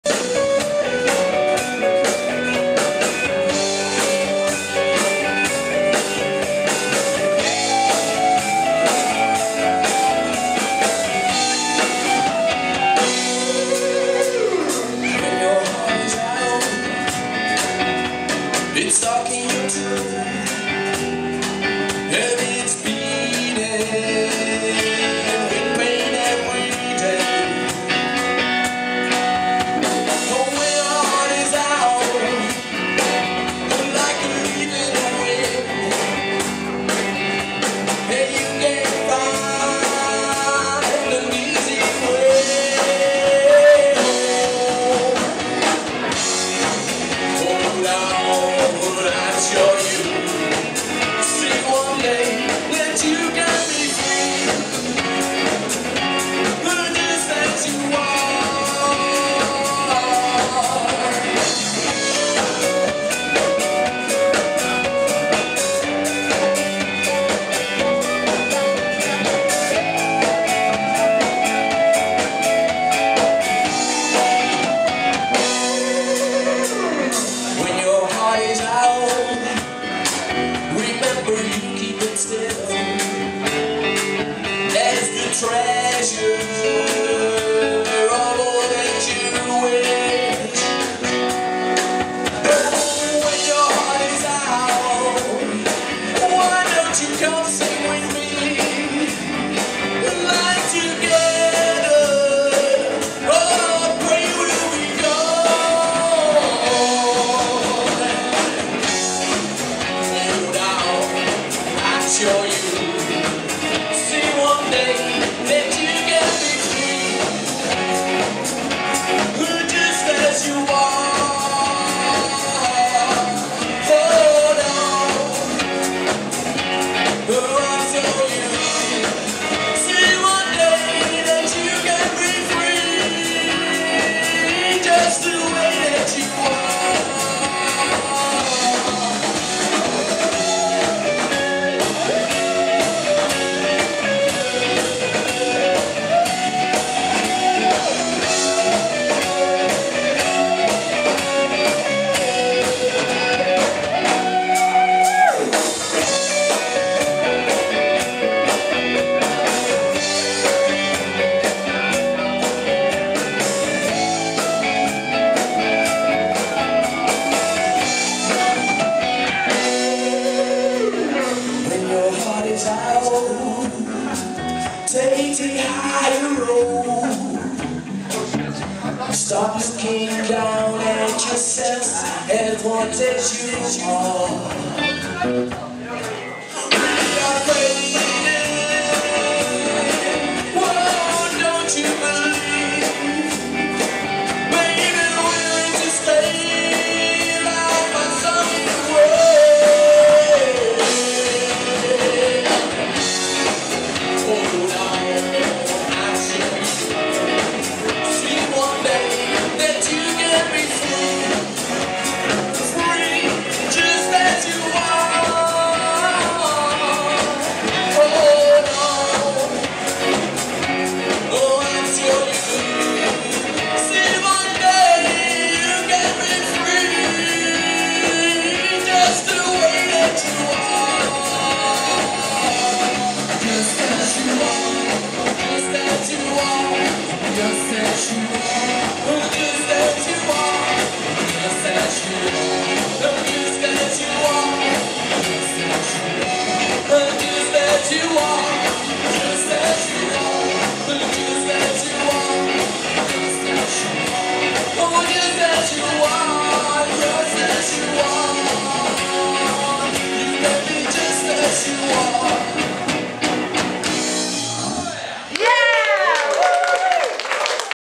''The Ease'' , Winnaar voorronde Kpv.Fryslân in Zero Dokkum
The Ease is een nieuwe band, bestaande uit ervaren muzikanten die persoonlijke nummers op een swingende en dansbare manier brengen.
Deelnemende bands waren deze avond : Bad End , The Exitlight Empire, Swahili Air Raid , Sweet God Animals , en de winnaar ! The Ease band uit Leeuwarden .